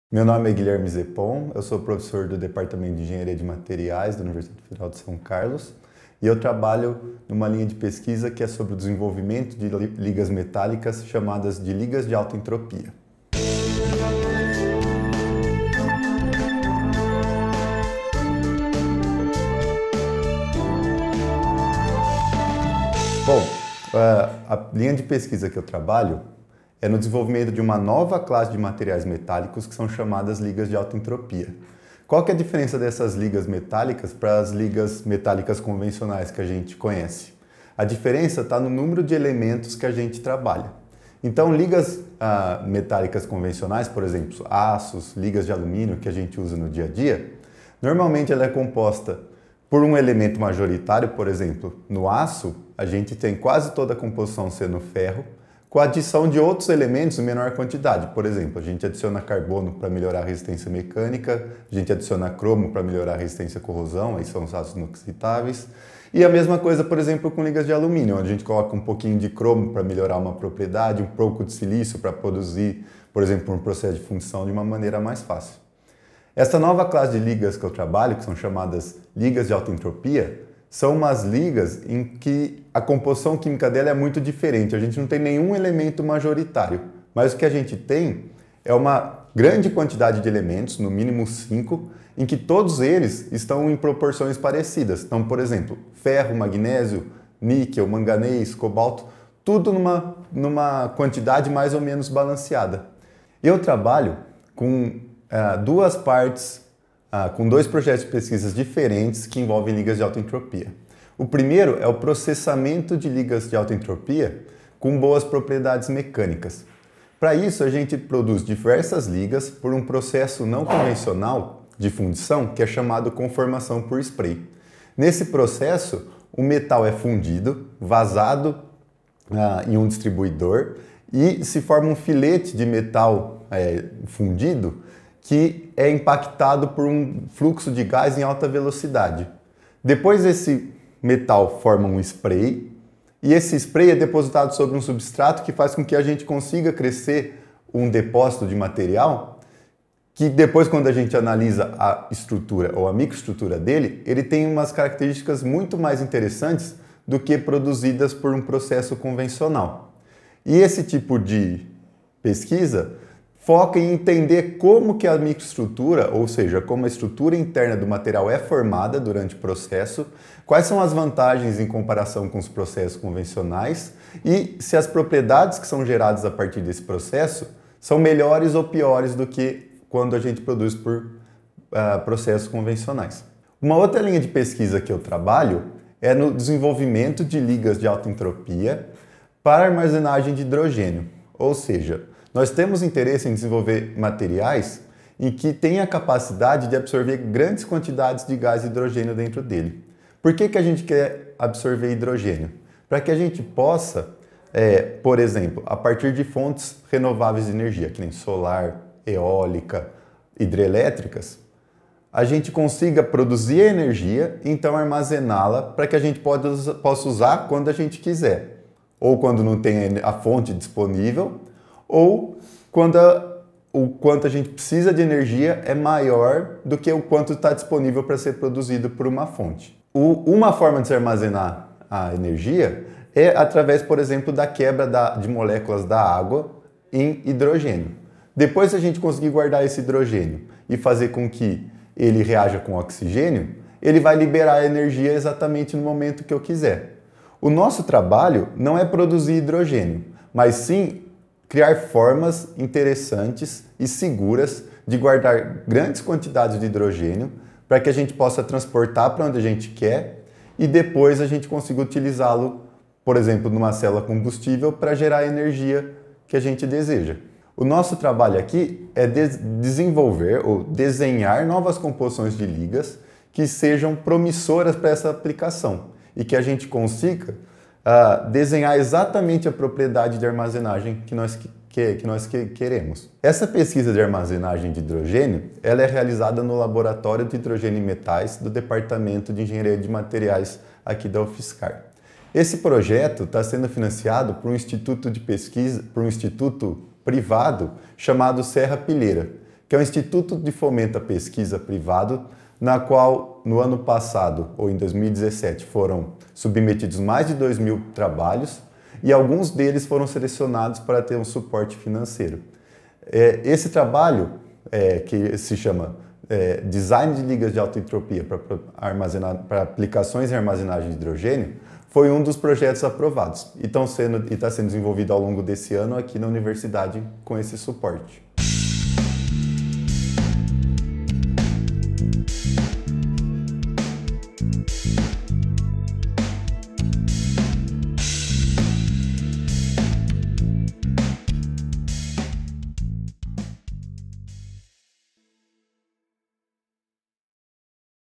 Ligas metálicas não convencionais são tema de pesquisas da UFSCar
Guilherme Zepon, professor do Departamento de Engenharia de Materiais da Universidade Federal de São Carlos (DEMa – UFSCar), fala de suas pesquisas com ligas metálicas não convencionais, chamadas de ligas de alta entropia.
CLICKCIÊNCIA